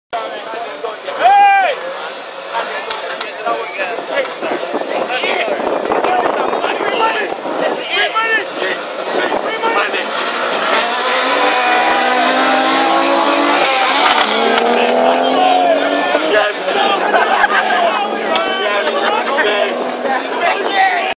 Lexus IS/ Toyota Altezza Drag
Drags at Bushy Park Barbados. 24/11/2012